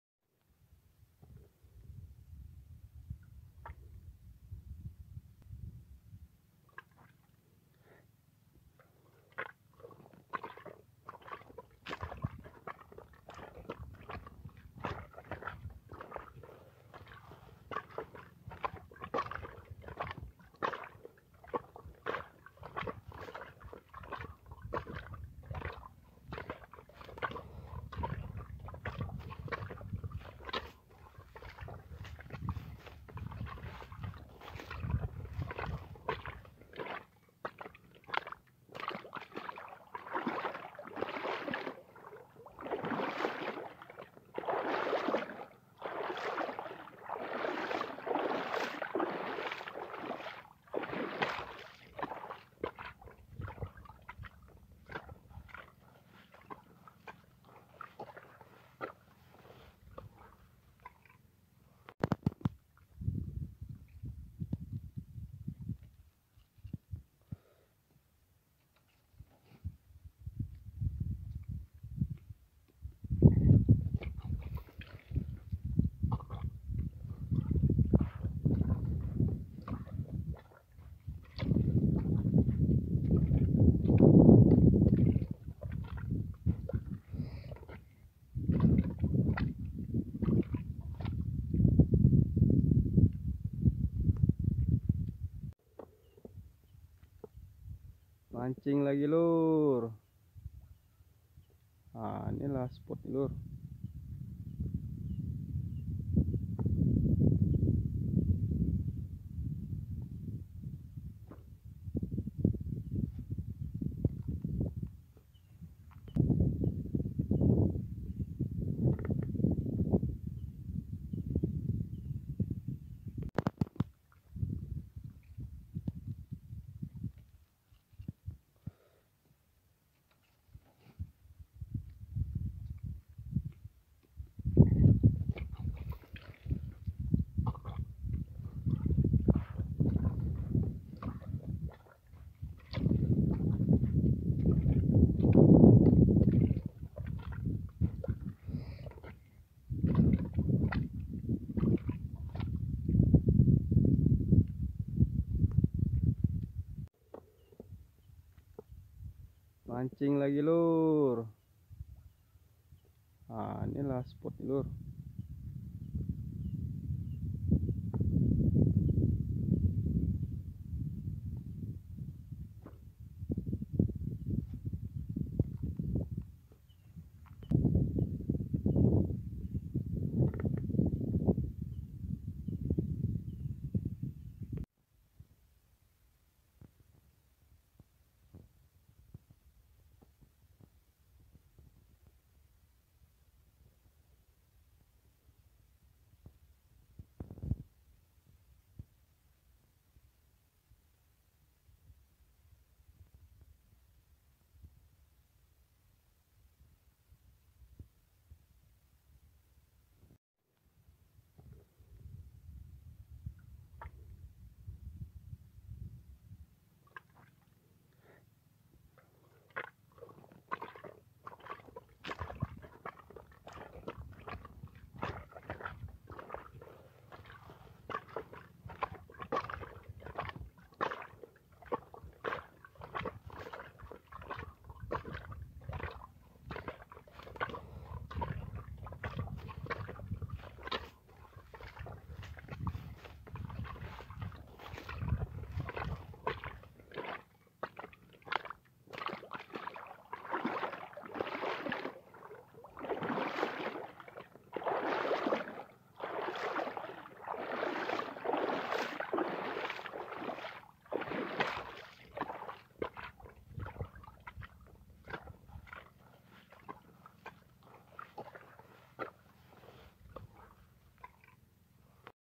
Review spot mancing, spot ok punya lur, spot anti boncoz
review spot mancing kali ini saya persembahkan buah kwan2 semua, spot nya ok punya lur, rawa artu.termasuk spot mancing betok, di sinu lah saya mancing.spot ini termasuk spot mancing semua jenis ikan, mancing betok pun sering kali saya di spot ini juga lur, , ,@Aan mlck